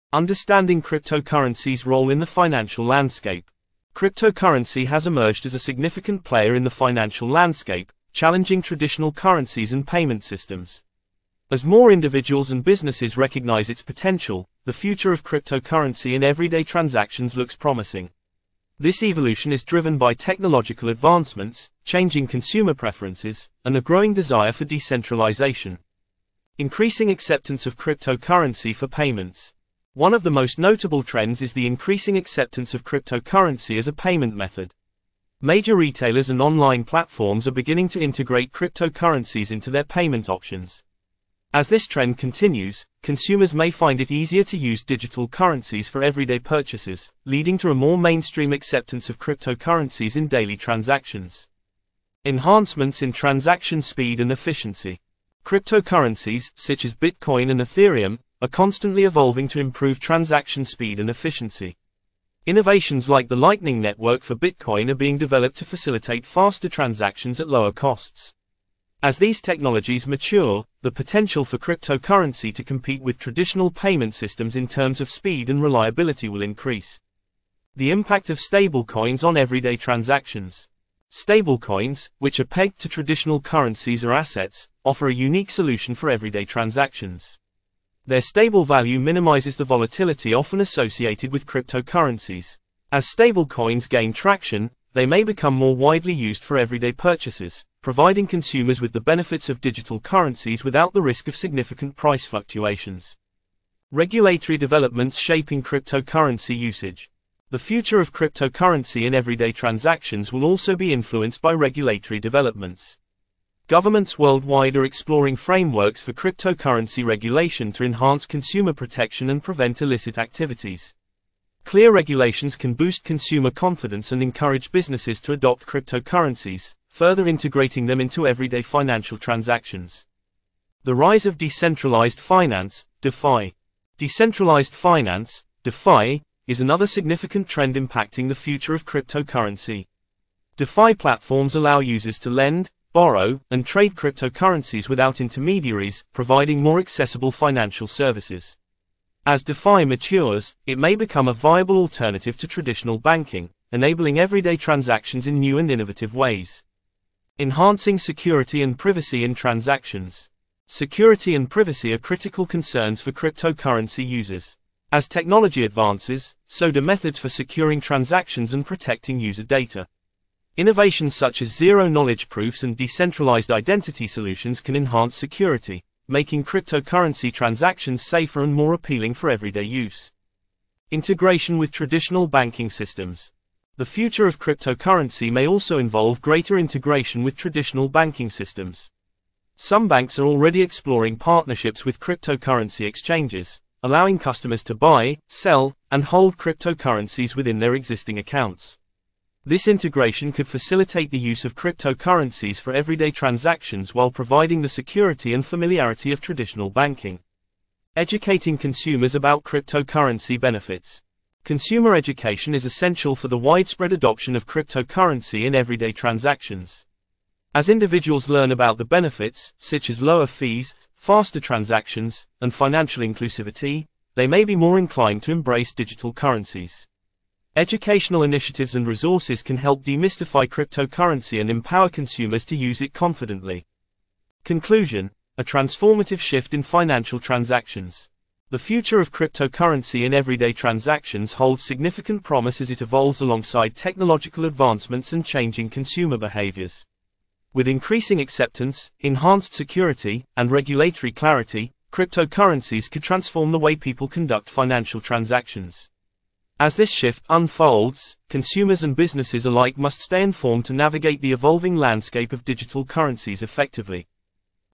What is the Future of Cryptocurrency in Everyday Transactions
Explore the future of cryptocurrency in everyday transactions. Will crypto become the new normal in shopping, investing, and banking?